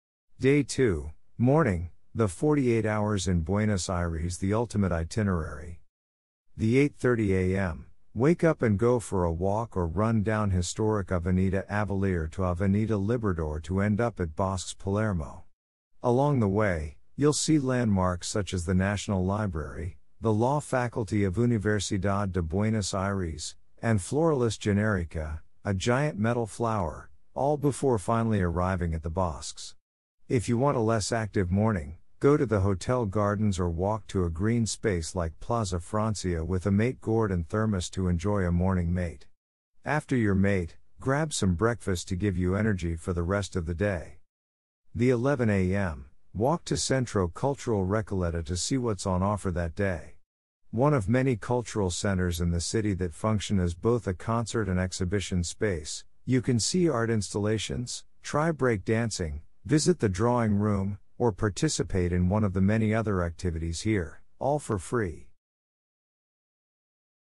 Day 2 Morning
Day 2 Morning - An episode of series: The 48 Hours in Buenos Aires The Ultimate Itinerary.
The 8:30 a.m.: Wake up and go for a walk or run down historic Avenida Avelear to Avenida Liberador to end up at Bosques Palermo. Along the way, you’ll see landmarks such as the National Library, the Law Faculty of Universidad de Buenos Aires, and Floralis Genérica (a giant metal flower), all before finally arriving at the Bosques. If you want a less active morning, go to the hotel gardens or walk to a green space like Plaza Francia with a mate gourd and thermos to enjoy a morning mate. After your mate, grab some breakfast to give you energy for the rest of the day..
The 11 a.m: Walk to Centro Cultural Recoleta to see what’s on offer that day. One of many cultural centers in the city that function as both a concert and exhibition space, you can see art installations, try break dancing, visit the drawing room, or participate in one of the many other activities here, all for free!.